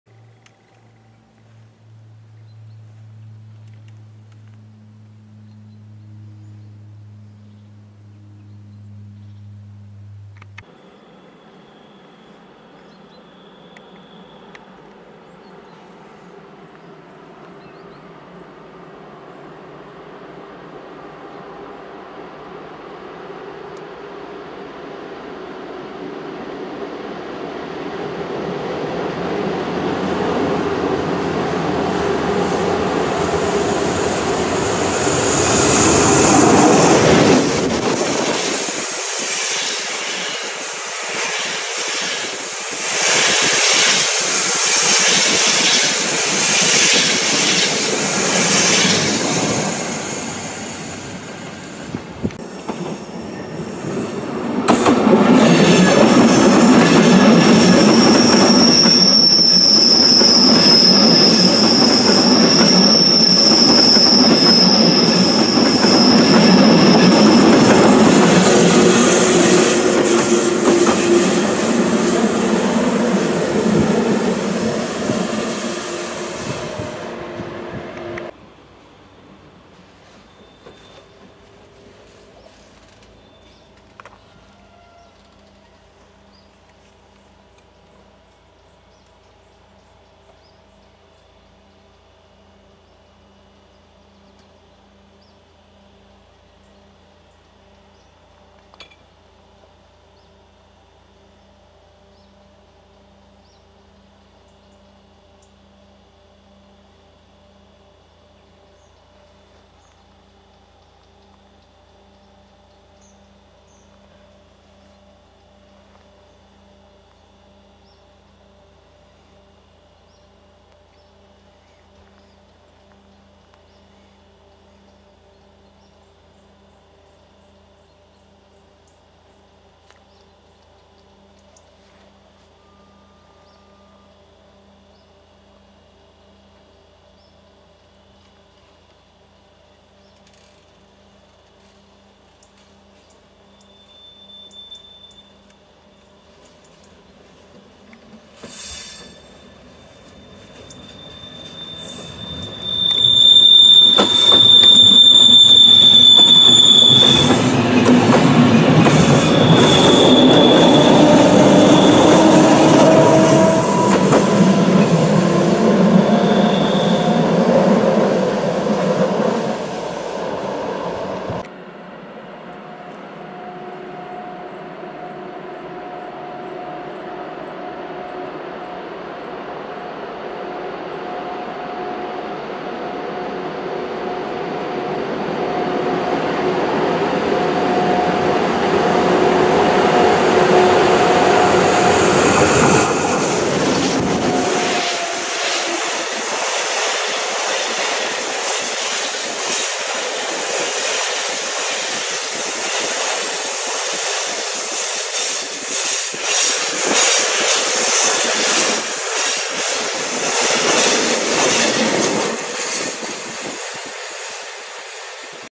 BAHNVERKEHR am WOLFSBERGKOGEL a.d. SEMMERINGBAHN am 25.08.2016 [FULL-HD]
Heute zusehen:"1142 mit Schnellzuggarnitur, RailJet, 6020 Regionalzug, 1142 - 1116 Doppeltracktion"